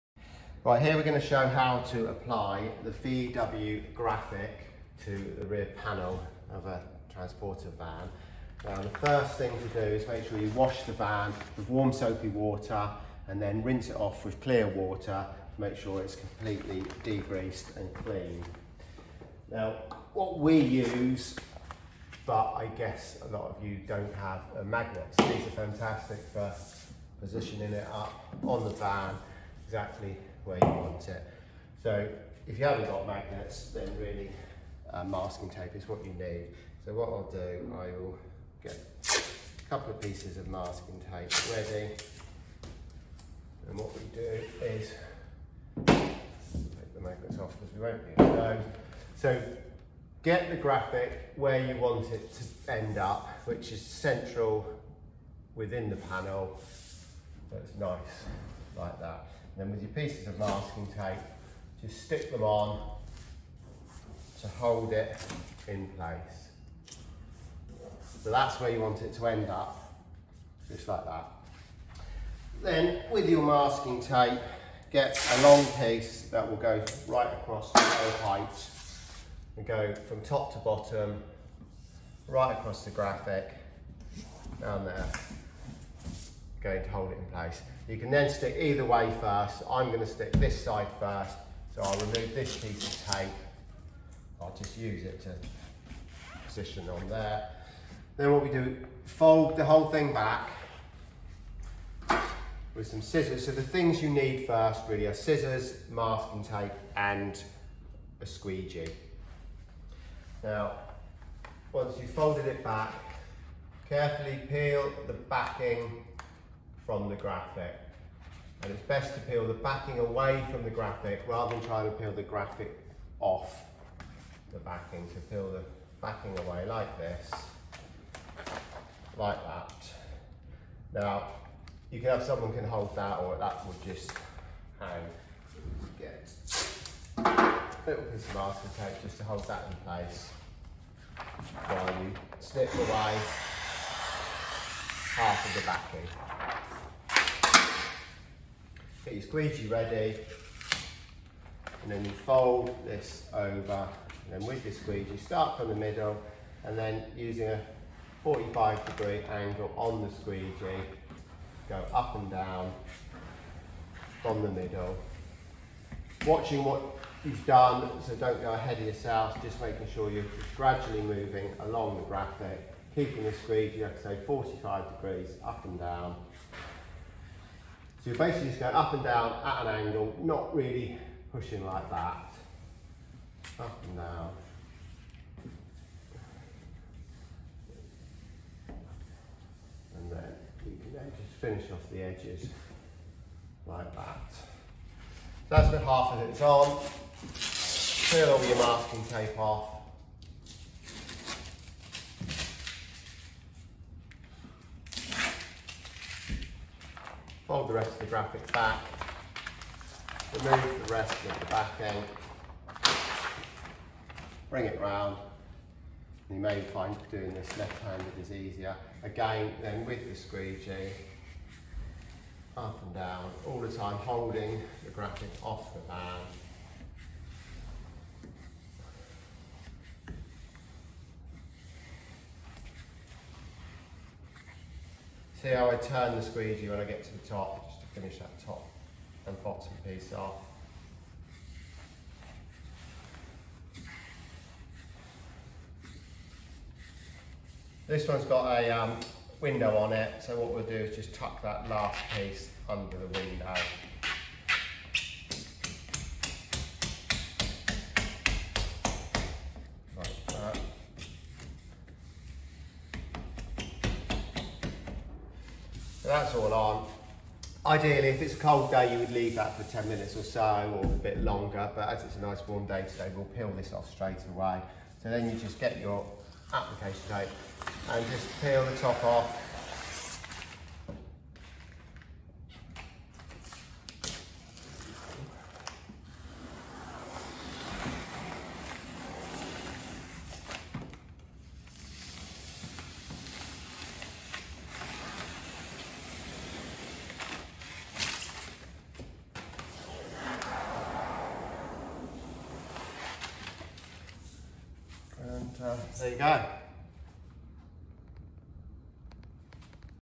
How to apply cool vinyl graphics to your VW camper van.
How to apply VW Camper van vinyl decals. Vinyl signs and graphics application.